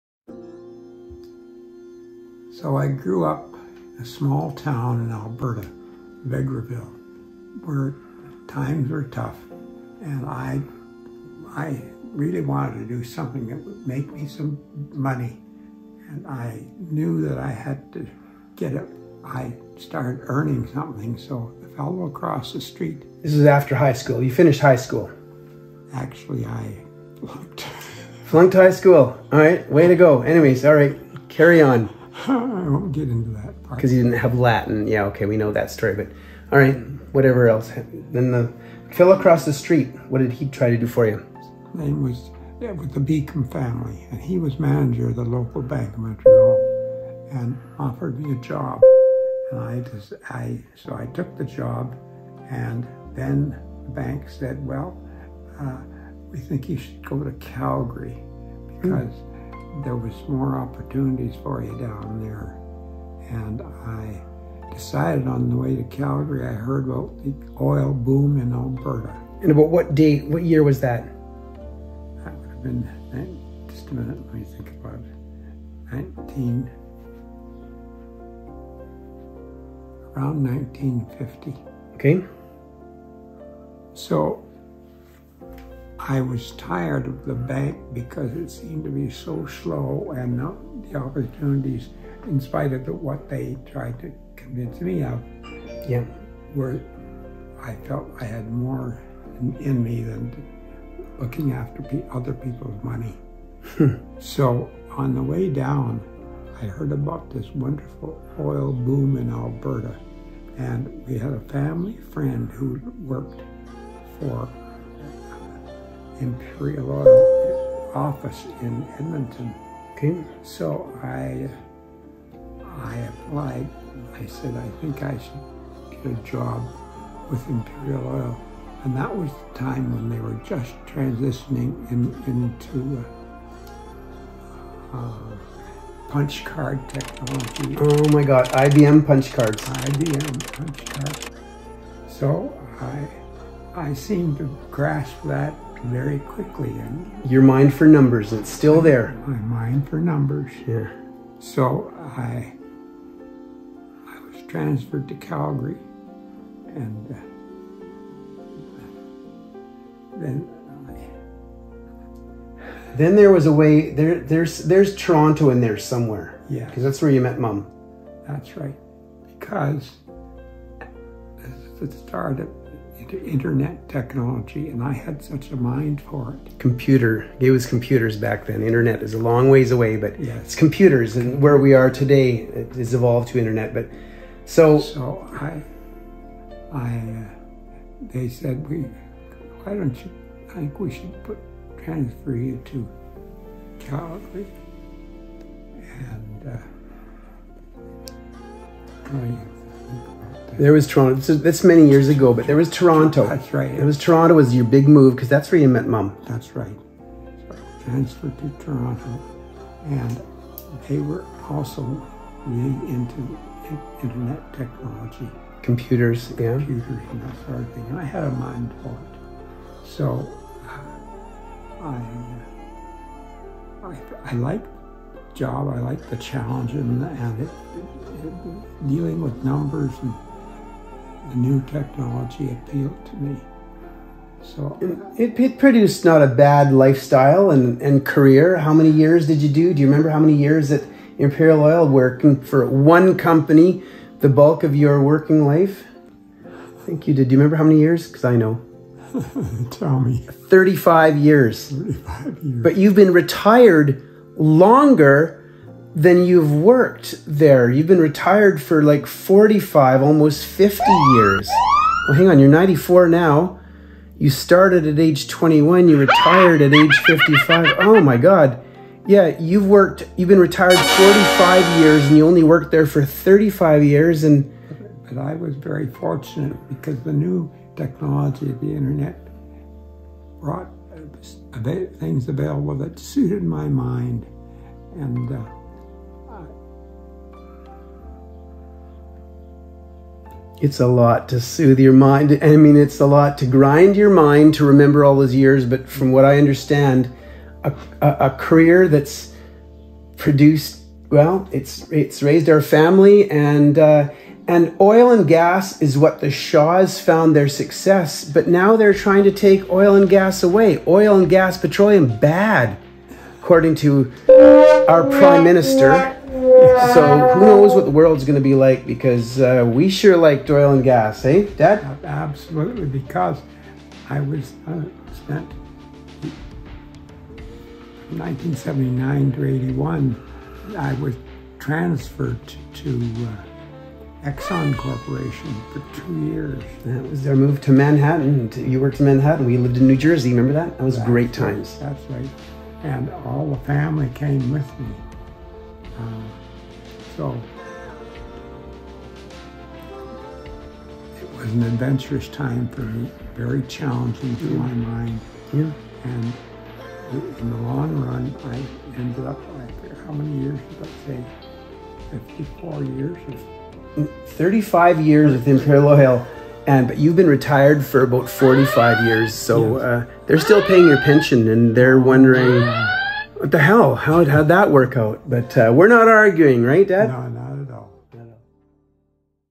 Gordon Shaw's Life & Career
Dad chats about his early life growing up in Vegreville, Alberta, Canada then choosing between banking and the oil and gas industry. Finally retiring as a Senior Executive with Imperial Oil they call each year to "check-in" 😉.  Hard not to understand our family's political leanings. 😂🙏✊🏼.